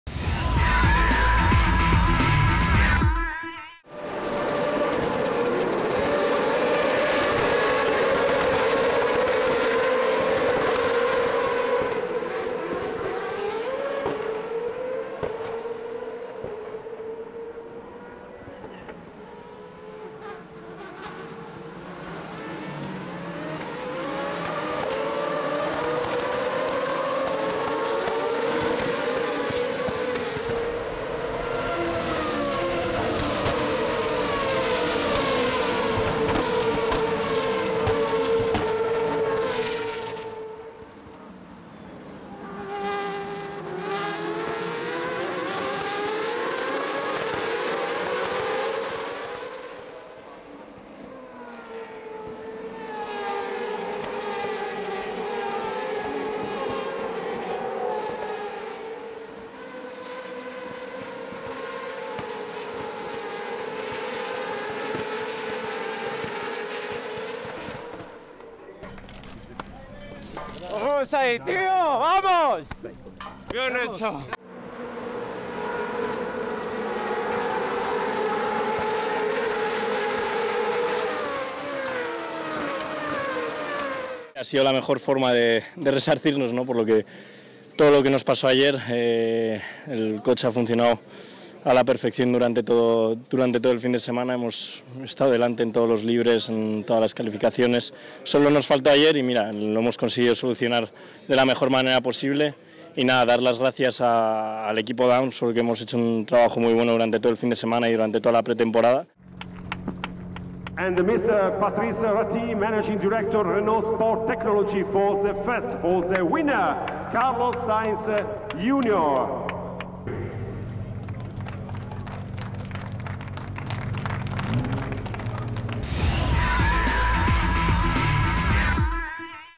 Monza 2014 - Formula Renault 3.5 Series - Race 2
On Sunday, Carlos Sainz (DAMS) took his first win in the Formula Renault 3.5 Series. Unlucky yesterday, the Red Bull Junior Team driver answered by dominating qualifying and the race on the Autodromo di Monza. He won ahead of Marlon Stöckinger (Lotus) and Sergey Sirotkin (Fortec Motorsports) and is now tied for the championship lead with Will Stevens (Strakka Racing) and Pierre Gasly (Arden Motorsport).

Ce dimanche, Carlos Sainz (DAMS) a signé sa première victoire en Formula Renault 3.5 Series. Malchanceux hier, le membre du Red Bull Junior Team a répondu en dominant les qualifications et la course sur l'Autodromo di Monza. Il a devancé Marlon Stöckinger (Lotus) et Sergey Sirotkin (Fortec Motorsports) au terme de la course, pour revenir à égalité de points avec Will Stevens (Strakka Racing) et Pierre Gasly (Arden Motorsport) en tête du classement général.